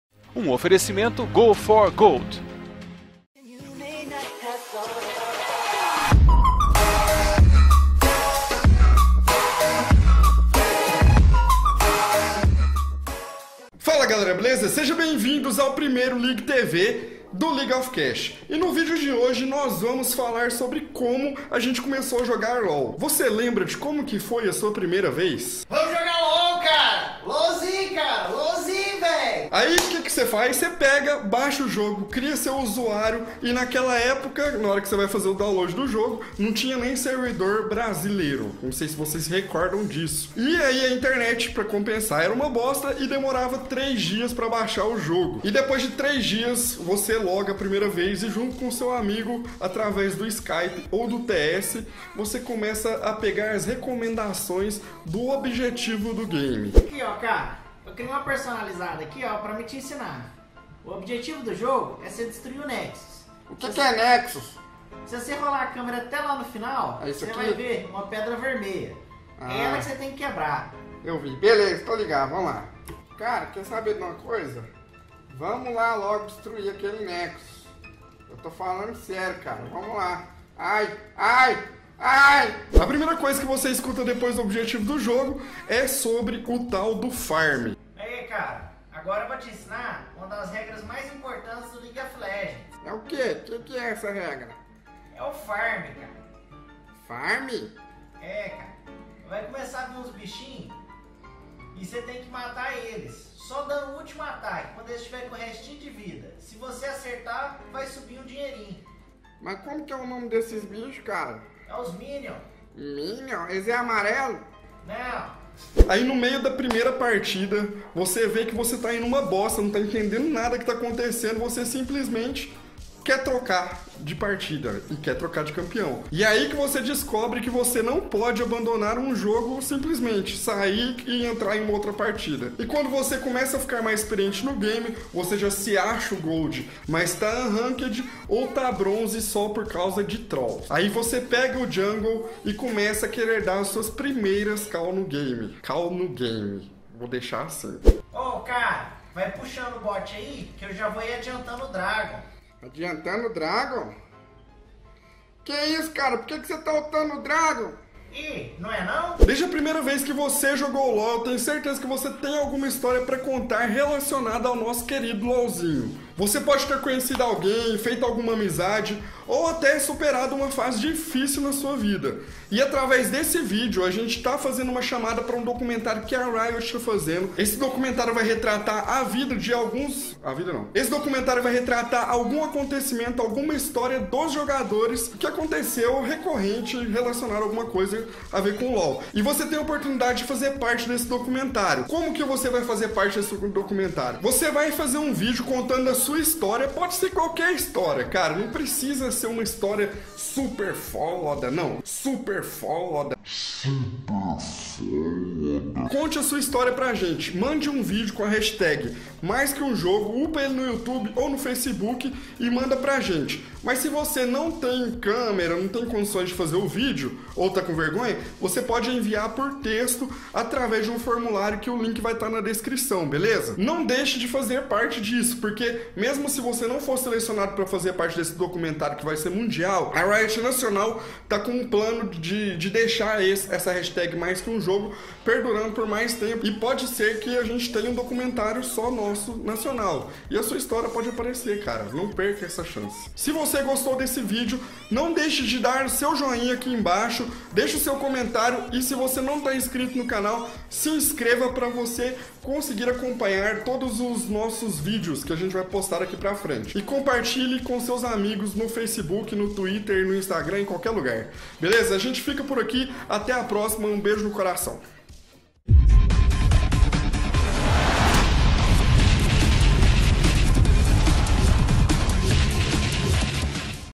League of Cast
Como foi sua primeira vez em "League of Legends"? Você apanhou muito? Não se preocupe, todos apanharam na primeira vez em um MOBA!

Você acha que "League of Legends" já deixou de ser apenas um jogo? Se você concorda com isso, saiba como participar do documentário oficial!